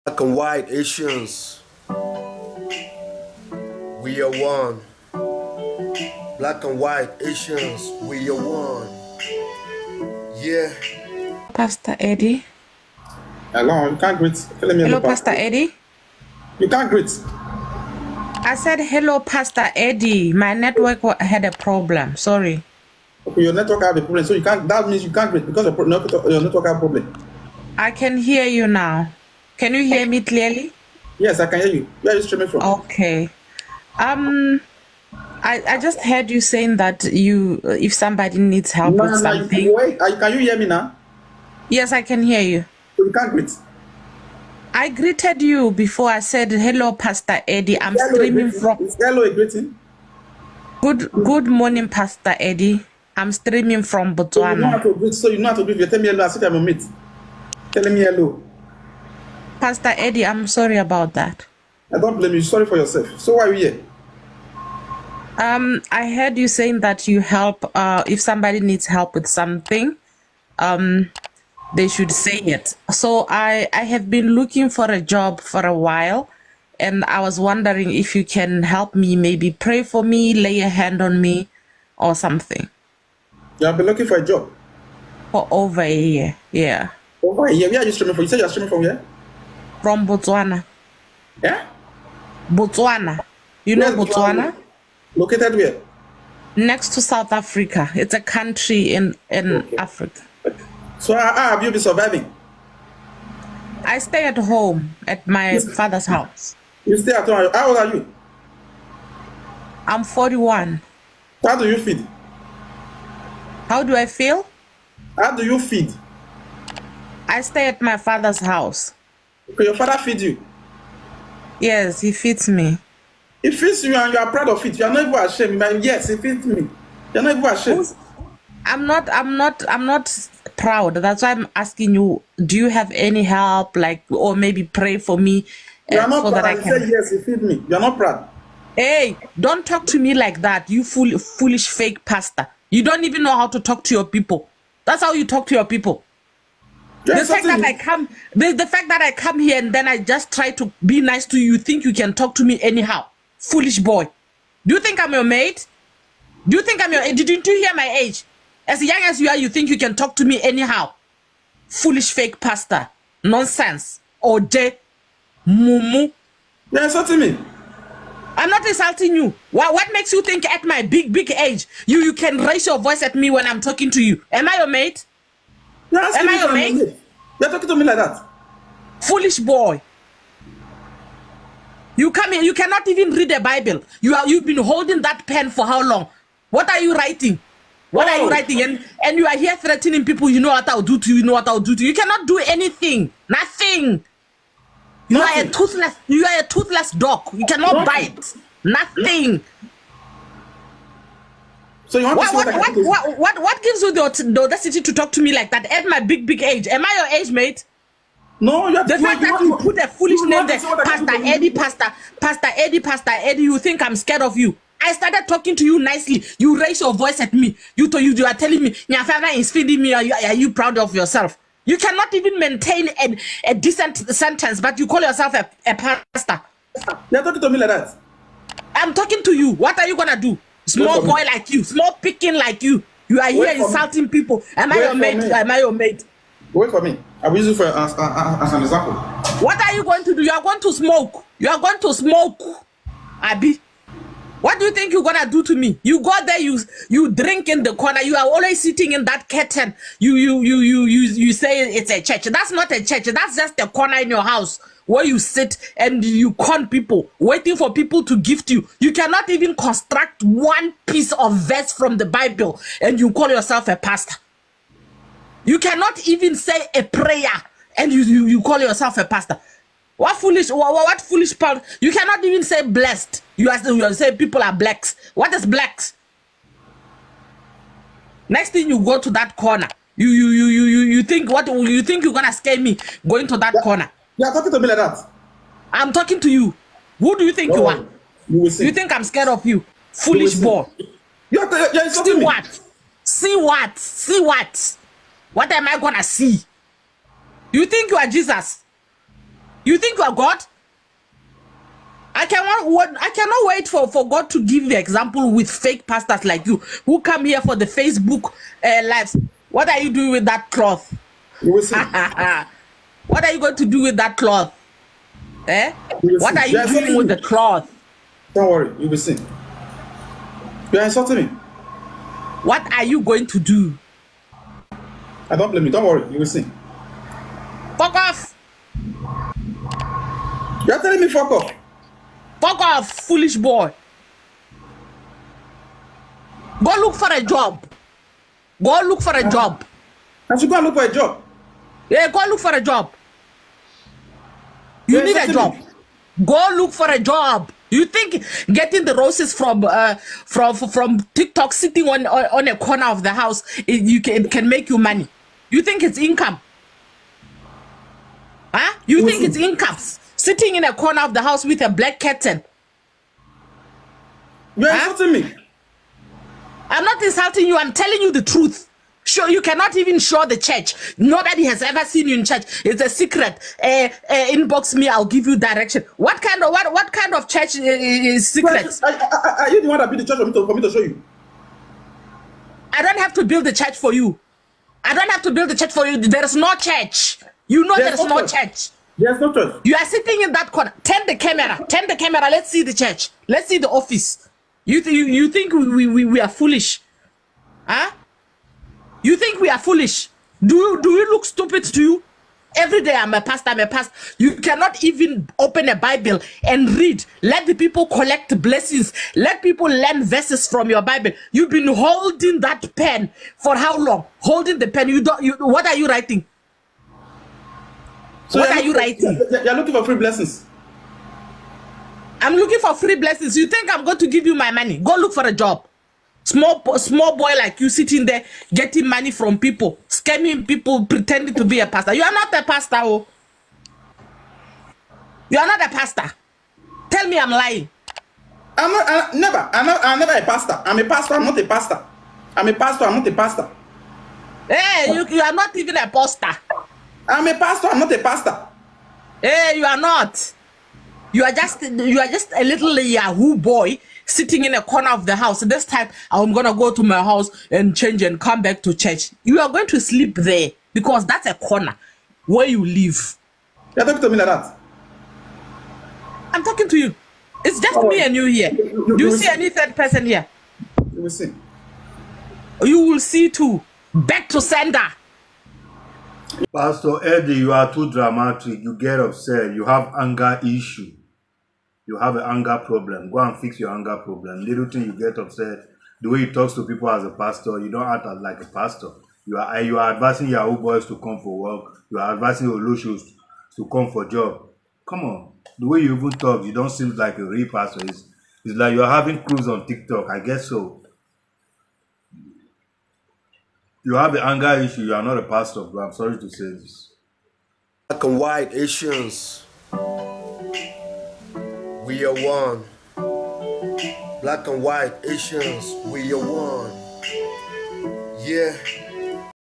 Botswana Lady Humiliate Pastor Eddy's On TikTok Live Pastor With Anger Issue. Funny 🤣😂
Pastor Eddy's Meet He's Match